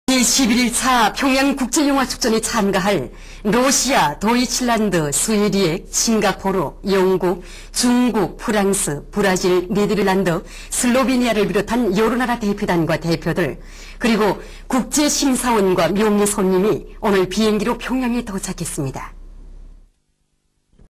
11th Pyongyang International Film Festival Delegation Arrive
Visits
   Pyongyang, September 16 (KCNA) --
Arriving on the same day to participate in the 11th Pyongyang International Film Festival were delegations and delegates of China, Singapore, Slovenia, Britain, Sweden, Germany, the Netherlands, Switzerland, France, Brazil and Italy and a Russian member of international jury for the festival and a German guest of honor.
    Earlier, a Mongolian delegation and Chinese, Tunisian and Indonesian members of international jury came here.